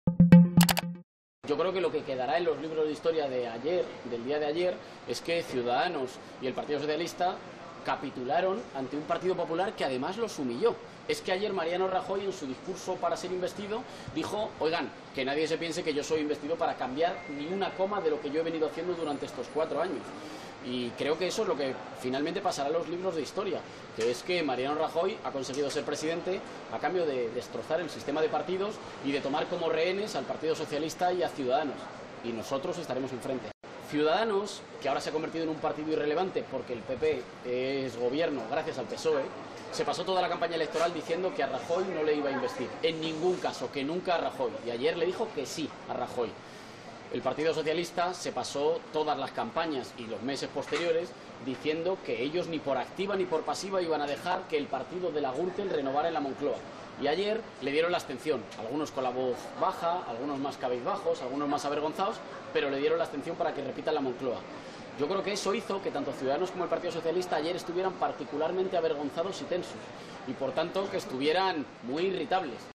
Íñigo Errejón: "PSOE y Ciudadanos capitularon ante un PP que les humilló"
Valencia, 30 oct (EFE).- El responsable de la Secretaría Política de Podemos, Íñigo Errejón, ha asegurado hoy que en la investidura de Mariano Rajoy se produjo una "infamia democrática" y lo que quedará para la historia es que PSOE y Ciudadanos (C's) "capitularon" ante un PP que "les humilló".

En declaraciones a los periodistas antes de participar en la inauguración de un nuevo centro social de la formación en Valencia, Errejón se ha referido así al debate de investidura de Rajoy, en el que, a su juicio, PSOE y C's "estuvieron particularmente avergonzados, tensos y muy irritables".

Frente a los rifirrafes y a las palabras que se cruzaron, el dirigente de Podemos cree que "en los libros de historia lo que quedará es que Ciudadanos y el Partido Socialista capitularon ante un PP que los humilló" porque Rajoy dijo en su discurso que no era investido "para cambiar ni una coma de lo que he venido haciendo durante estos cuatro años".

Ha recordado que el partido que lidera Albert Rivera, ahora "convertido en un partido irrelevante porque el PP gobierna gracias al PSOE", ayer dijo sí a Rajoy a pesar de que se pasó la campaña diciendo que "en ningún caso iba a investir a Rajoy".

"El PSOE se pasó la campaña y los meses posteriores diciendo que ellos ni por activa ni por pasiva iban a dejar que el partido de la Gürtel renovara en la Moncloa y ayer le dieron la abstención, algunos con la voz baja, algunos más cabizbajos, algunos más avergonzados", ha comentado. EFE

DECLARACIONES DEL RESPONSABLE DE LA SECRETARÍA POLÍTICA DE PODEMOS, ÍÑIGO ERREJÓN.